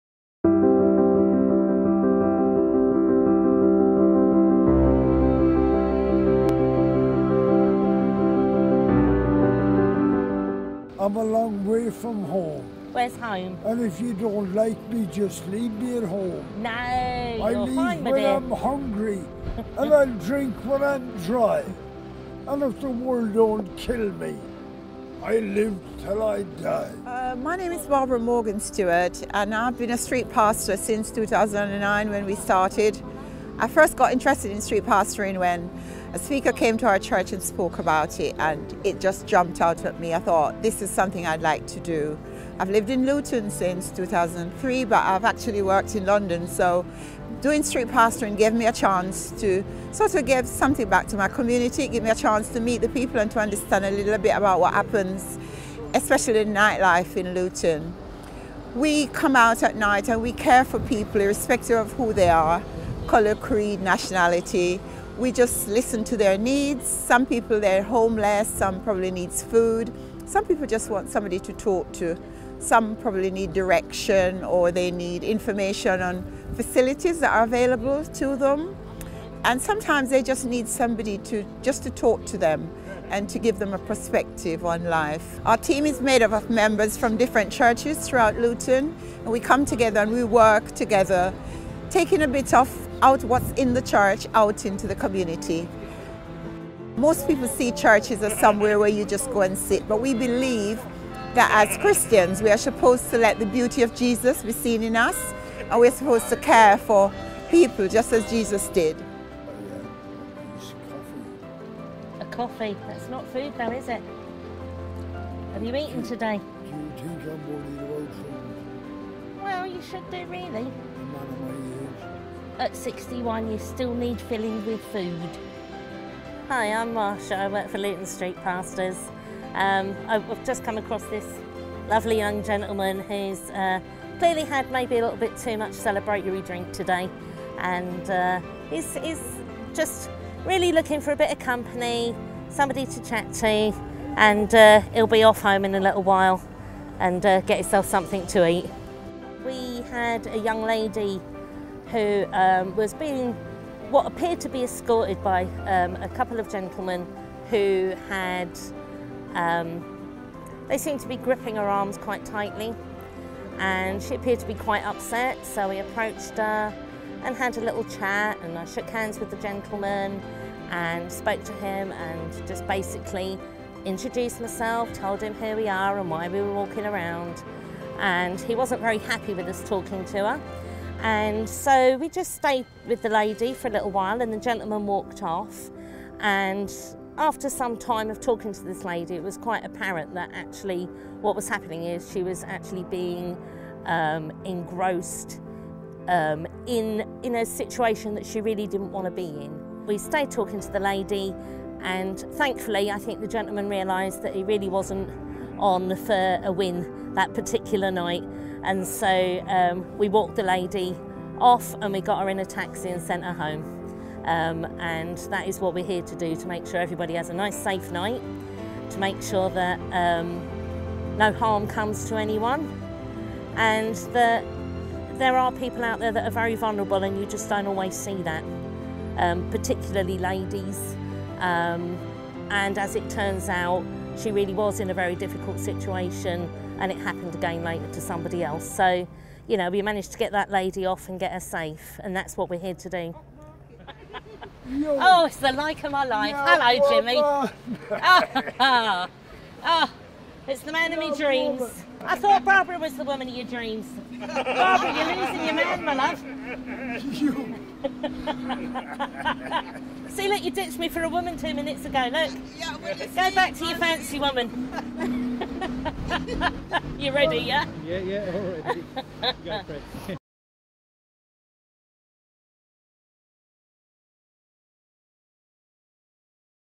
Bedfordshire High Sheriff Citizenship Award Winner: Luton Street Pastors
Bedfordshire High Sheriff Citizenship Award Winner